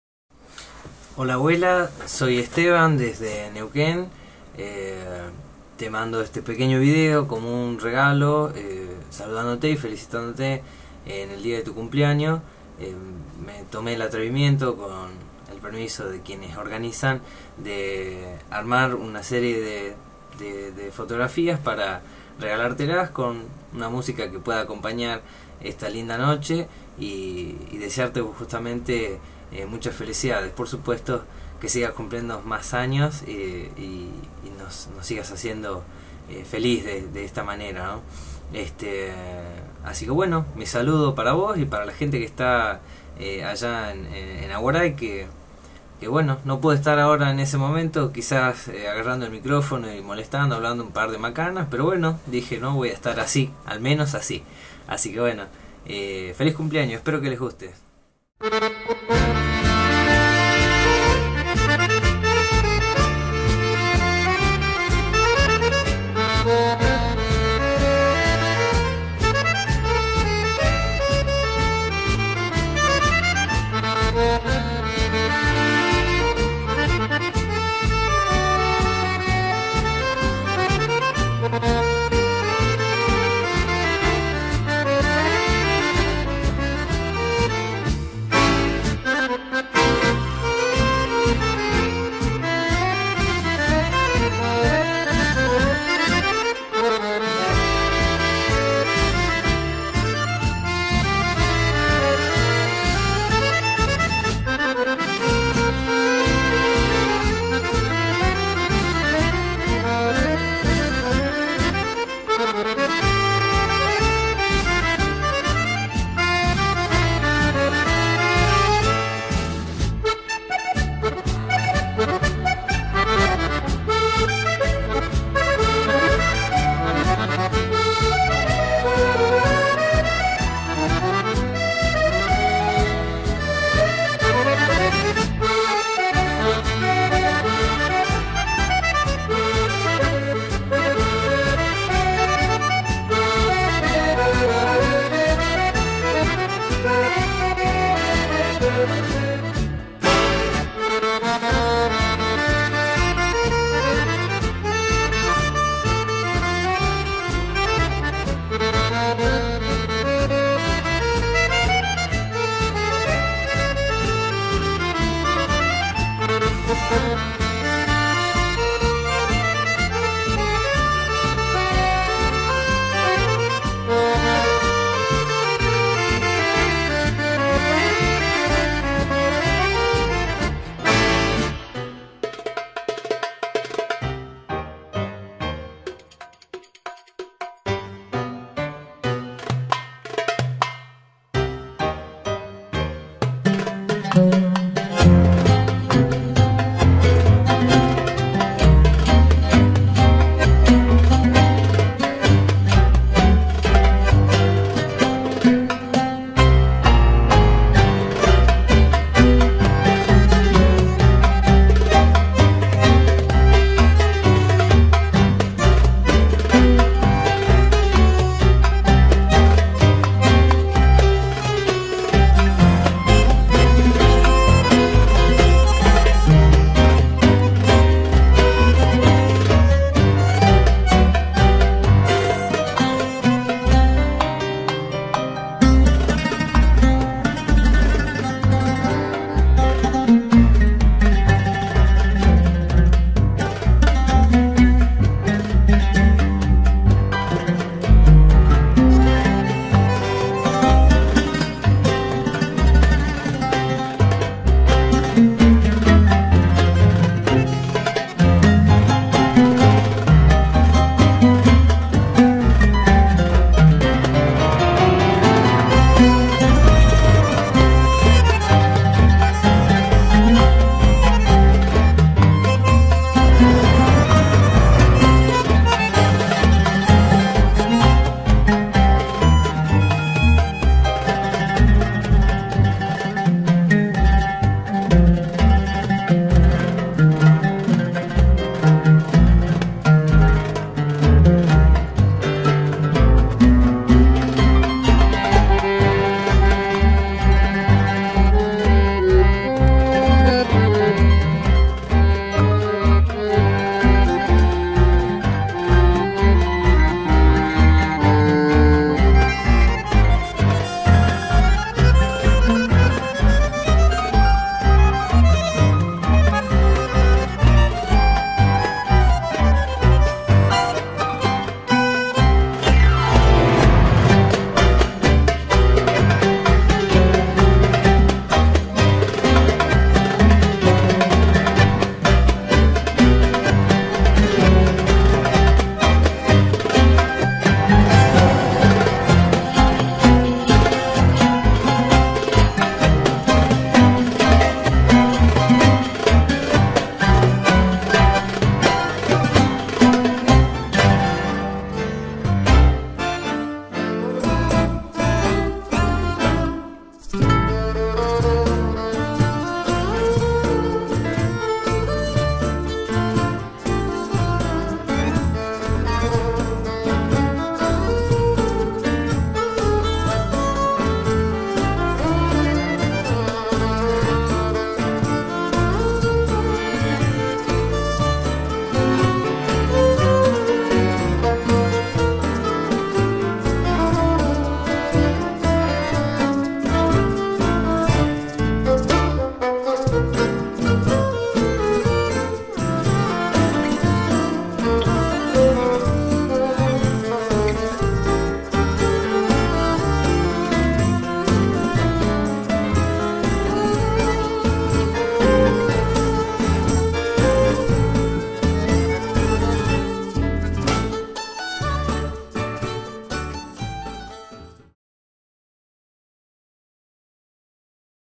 Cumpleaños "Tia Fanny Lahud" Aguaray-Salta
Video realizado para Fanny Lahud, más conocida en el pueblo de Aguaray por “la Tía Fanny”, declarada ciudadana ilustre.
En esta ocasión una emotiva recopilación de fotos de sus más lindos momentos para agasajar el día de su cumpleaños N° 80.